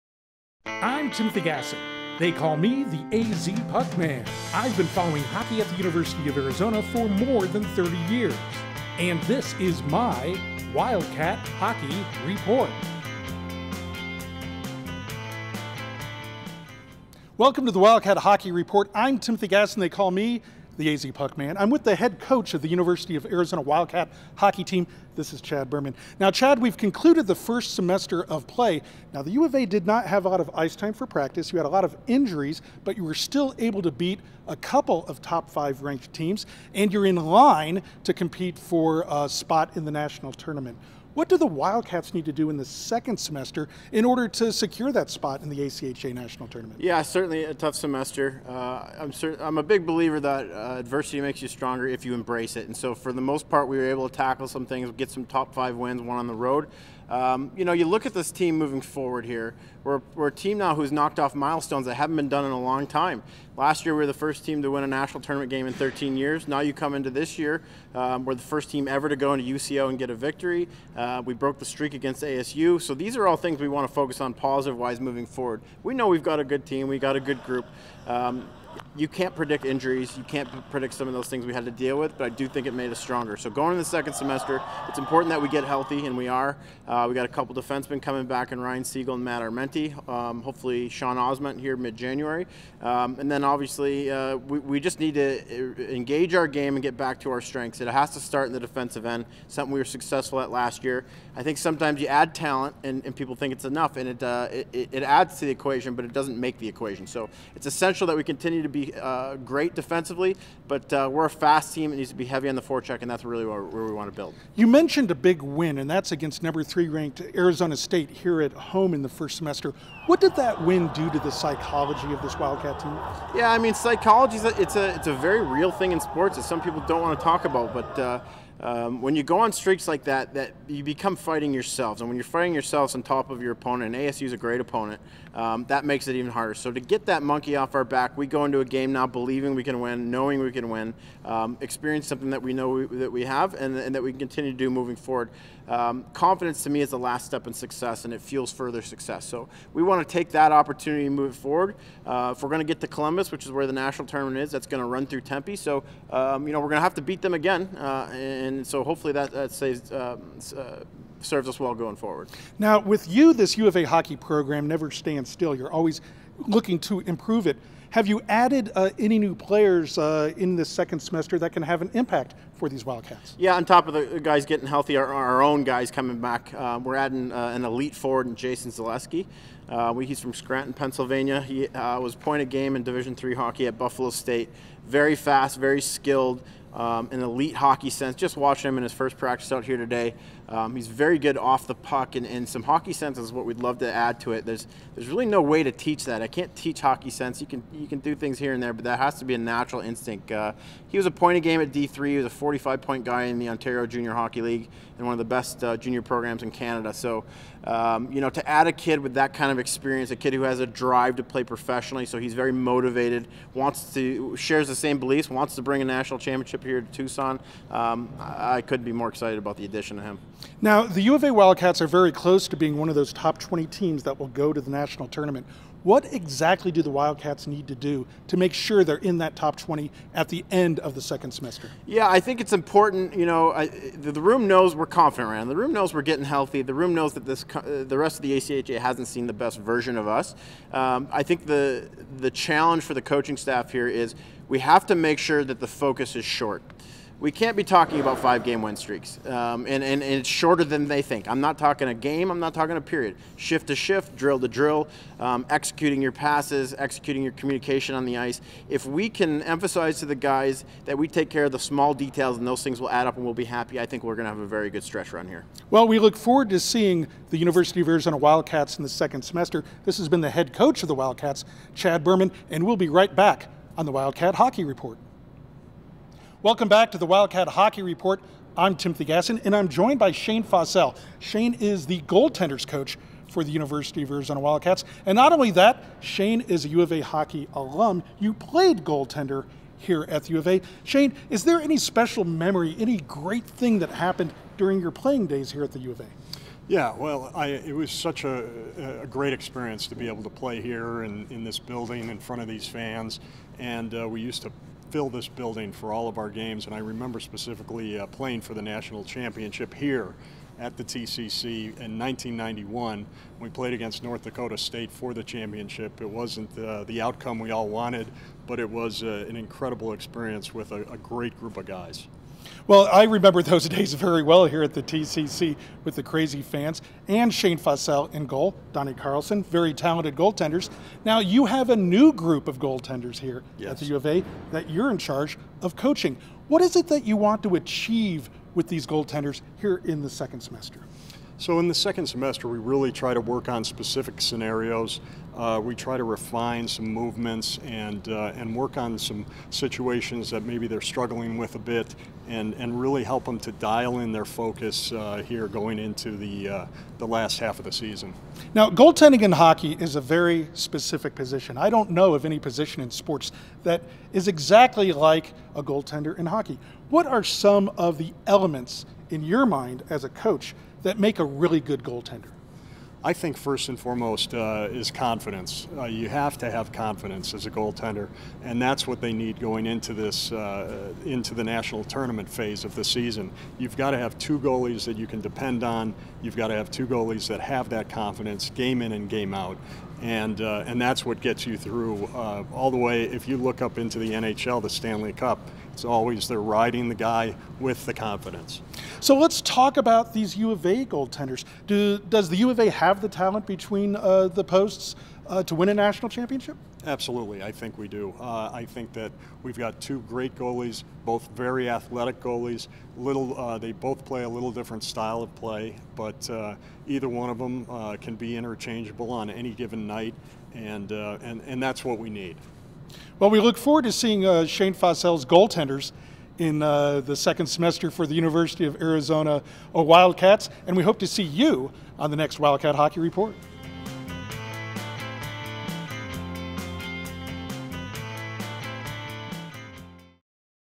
Wildcat Hockey Report #5 (January 2017)
University of Arizona Head Coach Chad Berman joins host Timothy Gassen for the fifth episode of the Wildcat Hockey Report -- following the U of A hockey team through the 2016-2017 season.